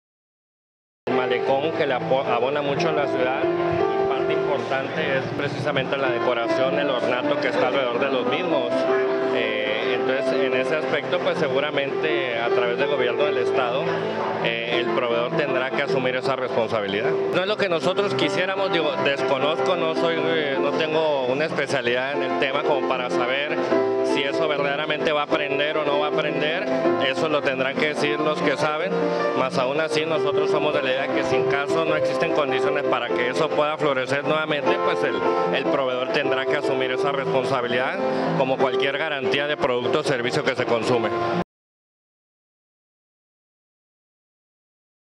Proveedores de palmeras deben asumir costos: Manguart
Con respecto al costo que ha generado la reposición de palmeras datileras en el malecón, el presidente de la Asociación de Hoteles Tres Islas, José Ramón Manguart Sánachez, señaló  que los proveedores tendrán que asumir la responsabilidad ya que la obra de remodelación, como cualquier otra debe tener una garantía. Dijo que el proyecto abona a una mejor imagen del puerto y parte importante es el ornato y decoración del paseo costero, aunque las palmeras no son la mejor imagen que se quisiera del malecón, será decisión del proveedor y el gobierno del estado qué procederá con esas palmeras que han sido traídas de otras parte de la República.